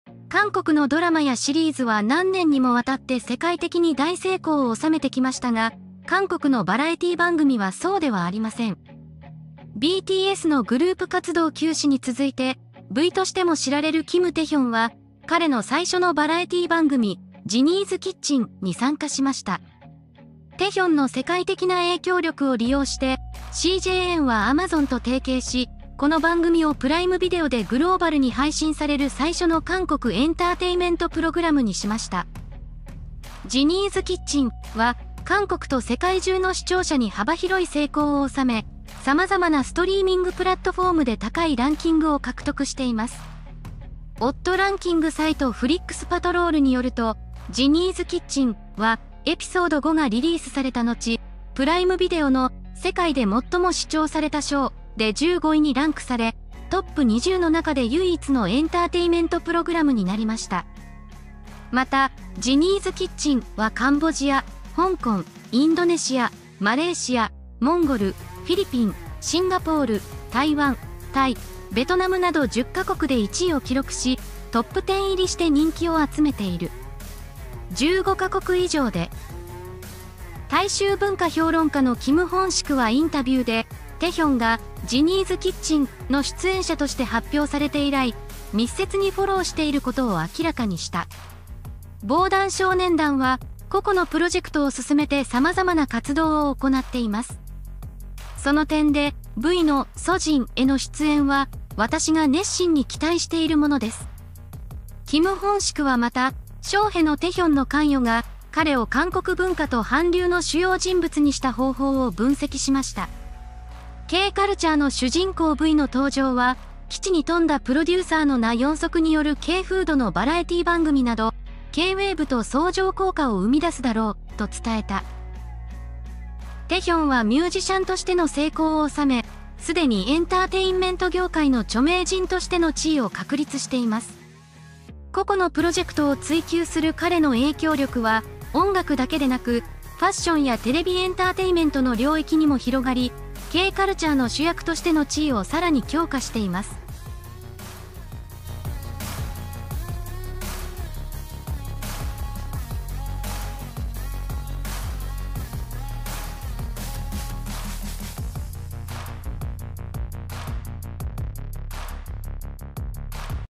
文化評論家のキム・ホンシクは、「ジニーのキッチン」の世界的な成功が高まる中、BTS の V キム・テヒョン を「K カルチャーの主人公」と呼んでいます。
韓国のドラマやシリーズは何年にもわたって世界的に大成功を収めてきましたが、韓国のバラエティ番組はそうではありません.

BTS のグループ活動休止に続いて、V としても知られるキム テヒョンは、彼の最初のバラエティ番組「ジニーズ キッチン」に参加しました。 テヒョンの世界的な影響力を利用して、CJ ENM は Amazon と提携し、この番組を Prime Video でグローバルに配信される最初の韓国エンターテイメント プログラムにしました。

「Jinny's Kitchen」は、韓国と世界中の視聴者に幅広い成功を収め、さまざまなストリーミング プラットフォームで高いランキングを獲得しています。

OTT ランキング サイト FlixPatrol によると、「ジニーズ キッチン」は、エピソード 5 がリリースされた後、Prime Video の「世界で最も視聴されたショー」で 15 位にランクされ、トップ 20 の中で唯一のエンターテイメント プログラムになりました。

また、「ジニーズキッチン」はカンボジア、香港、インドネシア、マレーシア、モンゴル、フィリピン、シンガポール、台湾、タイ、ベトナムなど10ヶ国で1位を記録し、トップ10入りして人気を集めている。 15カ国以上で。

大衆文化評論家のキム・ホンシクはインタビューで、テヒョンが「ジニーズキッチン」の出演者として発表されて以来、密接にフォローしていることを明らかにした。

「防弾少年団は、個々のプロジェクトを進めてさまざまな活動を行っています。その点で、Vの『ソジン』への出演は、私が熱心に期待しているものです。」

キム・ホンシクはまた、ショーへのテヒョンの関与が、彼を韓国文化と韓流の主要人物にした方法を分析しました.

Kカルチャーの主人公Vの登場は、機知に富んだプロデューサーのナ・ヨンソクによるKフードのバラエティ番組など、Kウェーブと相乗効果を生み出すだろう」と伝えた。

テヒョンはミュージシャンとしての成功を収め、すでにエンターテインメント業界の著名人としての地位を確立しています。

個々のプロジェクトを追求する彼の影響力は、音楽だけでなく、ファッションやテレビ エンターテイメントの領域にも広がり、K カルチャーの主役としての地位をさらに強化しています。